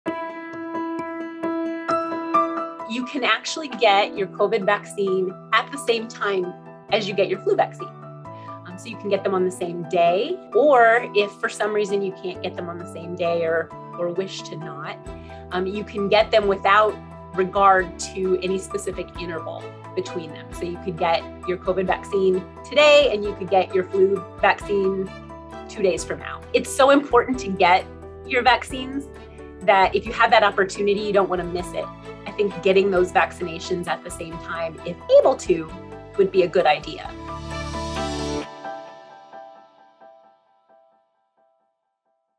Ask an Expert: Should I get the flu shot and the COVID-19 booster at the same time?
Dr. Lori Rolando shares the latest guidance on when to get the flu shot and the COVID-19 booster shot. Both can be received simultaneously and with no wait time in between vaccines. If the booster shot is not yet recommended for your demographic, it is important to get the flu shot as soon before the end of October, and the booster shot when it becomes available or recommended. Dr. Rolando is the director of the Occupational Health Clinic at Vanderbilt University Medical Center.